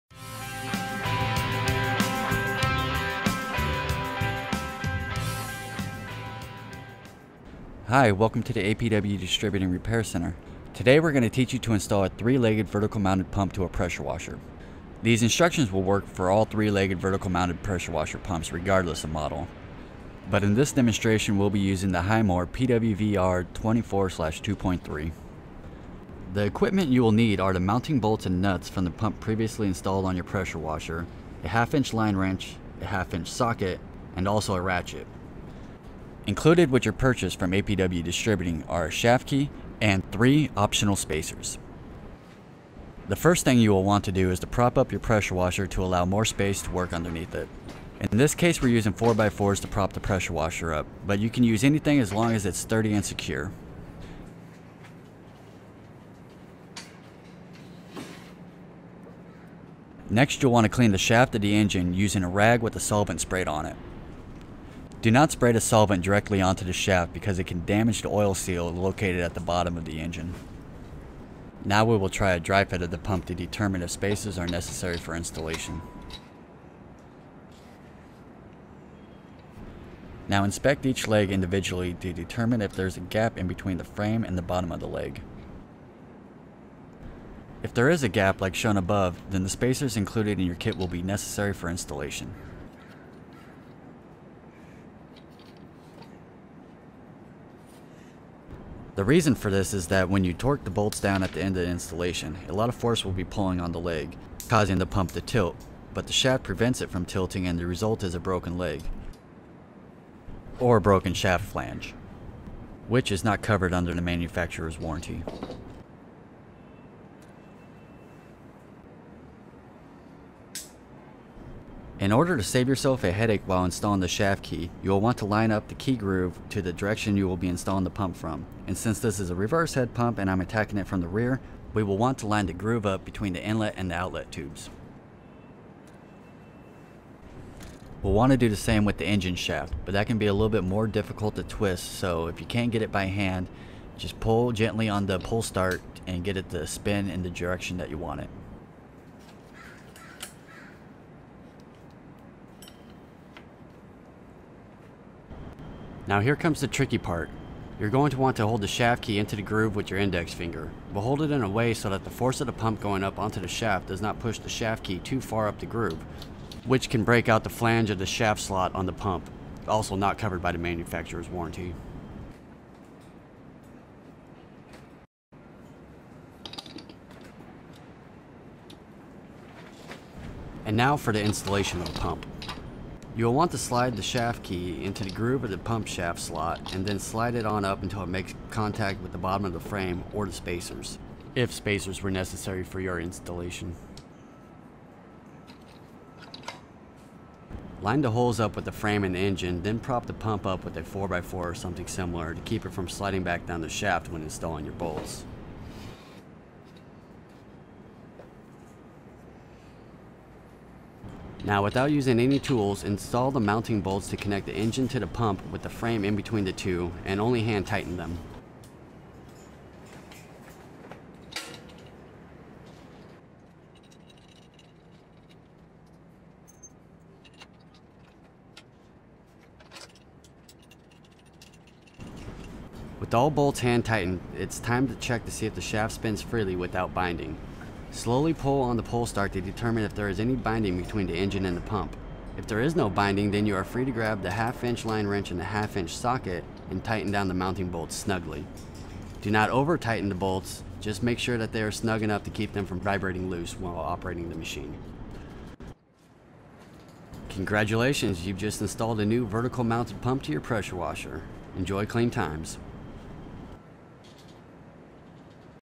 How to install a vertical pressure washer pump
Today we're going to teach you how to install a 3-legged vertical mounted pump to a pressure washer. These instructions will work for all 3-legged vertical mounted pressure washer pumps, regardless of the model, but in this demonstration we'll be using a Himore PWVR24/2.3. The equipment you will need are the mounting bolts and nuts from the pump previously installed on your pressure washer, a half inch line wrench, a half inch socket and also a ratchet. Included with your purchase from APW Distributing are a shaft key and 3 optional spacers. the first thing you'll want to do is prop up your pressure washer to allow more space to work underneath it, In this case we're using 4x4's to hold the pressure washer up, but you can use anything as long as it's sturdy and secure. Next, you'll want to clean the shaft of the engine using a rag with a solvent sprayed on it. DO NOT spray the solvent directly onto the shaft because it can damage the oil seal located at the bottom of the engine. Now, we will try a dry fit of the pump to determine if spacers are necessary for installation. Now inspect each leg individually to determine if there is a gap between the frame and the bottom of the leg. If there is a gap, like shown above, then the spacers included in your kit will be necessary for installation. The reason for this is that when you torque the bolts down at the end of installation a lot of force will be pulling on the leg, causing the pump  to tilt, but the shaft prevents it from tilting and the result is a broken leg or a broken shaft flange, which is not covered under the manufacturer's warranty. In order to save yourself a headache while installing the shaft key you will want to line up the key groove to the direction you will be installing the pump from. since this is a reverse head pump and I'm attacking it from the rear, we want to align the groove up between the inlet and outlet tubes. We'll want to do the same with the engine shaft, but that can be a little bit more difficult to twist. So, if you can't get it by hand, just pull gently on the pull start and get it to spin to the direction you want it. Now, here comes the tricky part: you're going to want to hold the shaft key into the groove with your index finger. hold it in a way so that the force of the pump going onto the shaft does not push the shaft key too far up the groove, which can break out the flange of the shaft slot on the pump, also not covered by the manufacturer's warranty. And now for the installation of the pump: you will want to slide the shaft key into the groove of the pump shaft slot, and slide it on up until it makes contact with the bottom of the frame or the spacers if spacers were necessary for your installation. Line the holes up with the frame and engine, then prop the pump up with a 4x4 or something similar to keep it from sliding back down the shaft when installing your bolts. Now, without using any tools, install the mounting bolts that connect the engine to the pump with the frame between the two, and only hand tighten them. With all the bolts hand tightened, it's time to check to see if the shaft spins freely without binding. If there is no binding, then you are free to grab the half inch line wrench and the half inch socket and tighten down the mounting bolts down snugly. Do not over-tighten the bolts, just make sure that they are snug enough to keep them from vibrating loose while operating the machine.